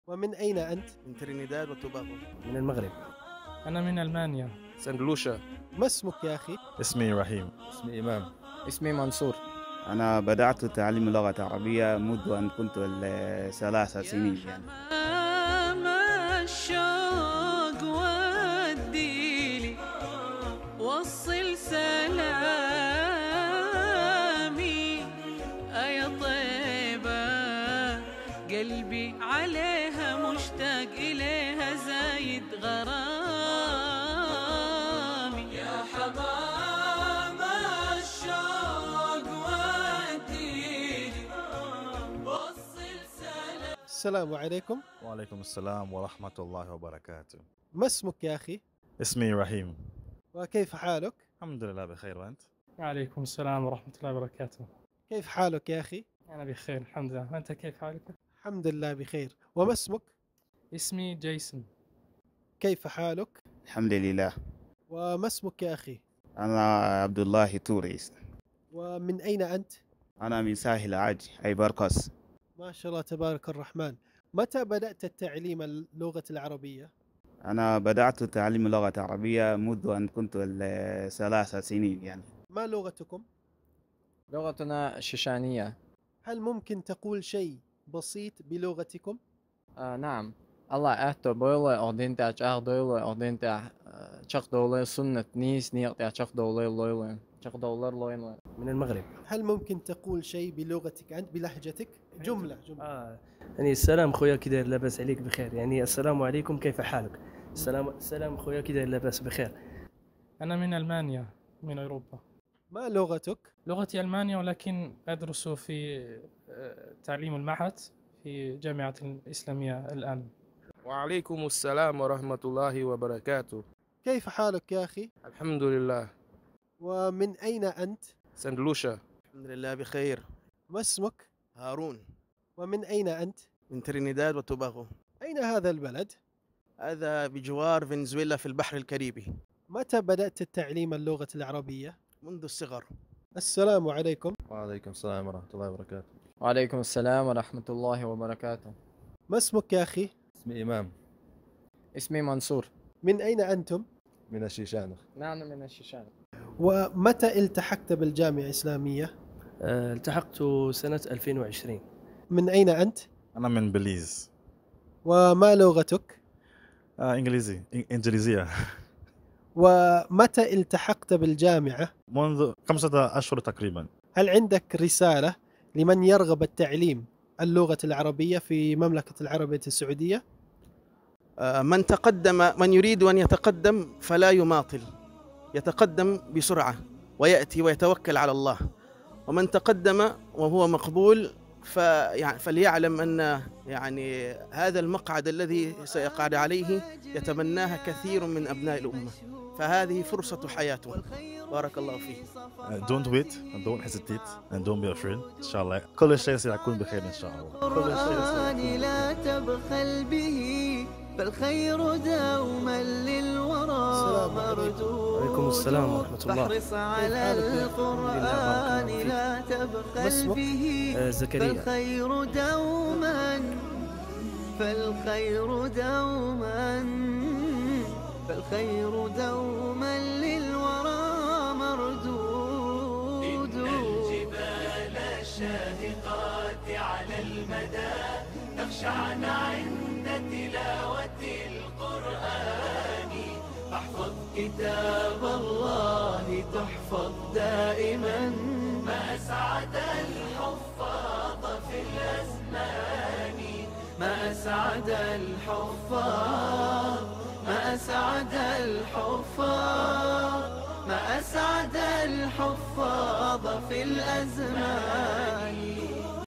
Basic Arabic conversation with students from the Islamic University of Medinah
A diverse group of students from around the globe learning Arabic as a second language, each with their own unique story and perspective on studying Arabic.